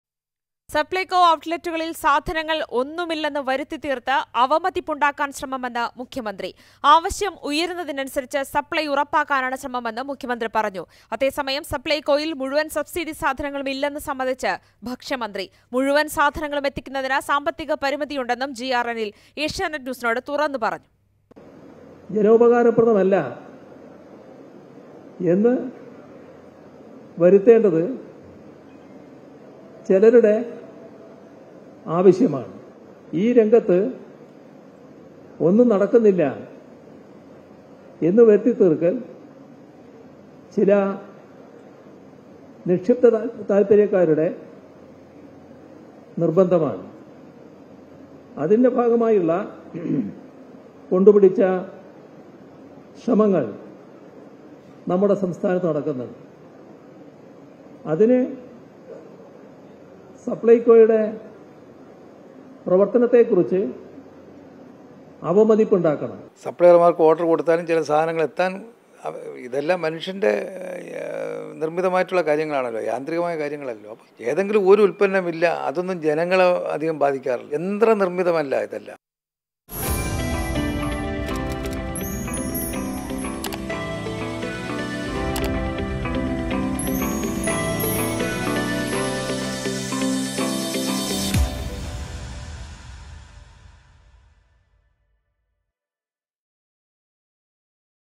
സാധനങ്ങൾ ഇല്ലെന്ന് വരുത്തി തീർത്ത് അവമതിപ്പുണ്ടാക്കാൻ ശ്രമമെന്ന് മുഖ്യമന്ത്രി | Supplyco | Scarcity
സപ്ലൈകോ ഔട്ട്ലെറ്റിൽ സാധനങ്ങൾ ഒന്നും ഇല്ലെന്ന് വരുത്തി തീർത്ത് അവമതിപ്പുണ്ടാക്കാൻ ശ്രമമെന്ന് മുഖ്യമന്ത്രി; സപ്ലൈകോയിൽ മുഴുവൻ സാധനങ്ങളുമില്ലെന്ന് സമ്മതിച്ച് ഭക്ഷ്യമന്ത്രി
Supplyco | Scarcity | CM Pinarayi Vijayan